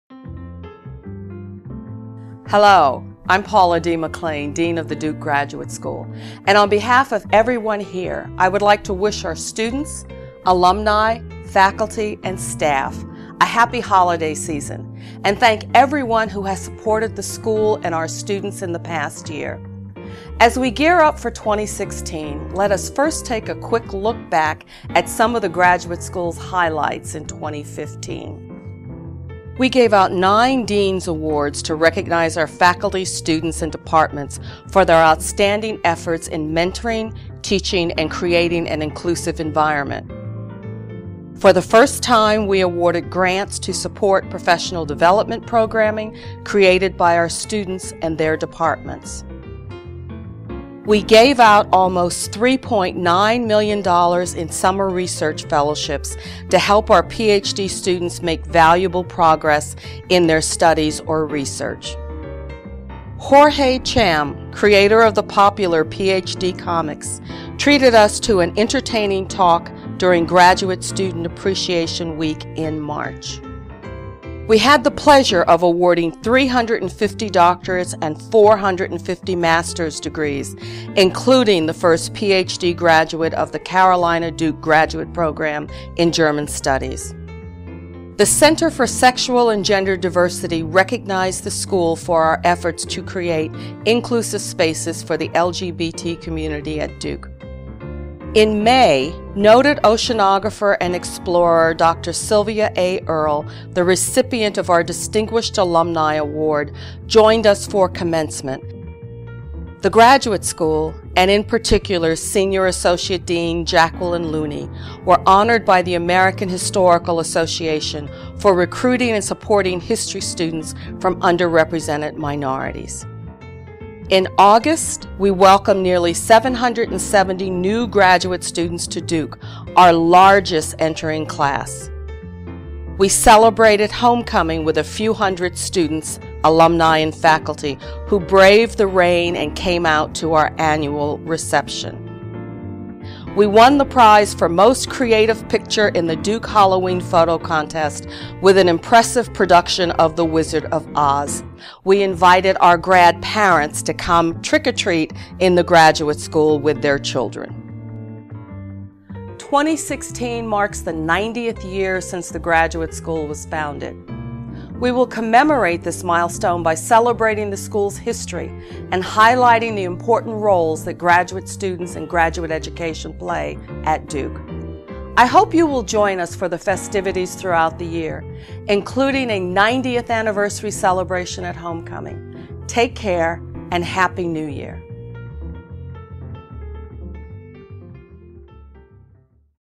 Happy Holidays from The Graduate School
A holiday message from Dean Paula D. McClain, with a look back at highlights from 2015 and a look ahead to 2016.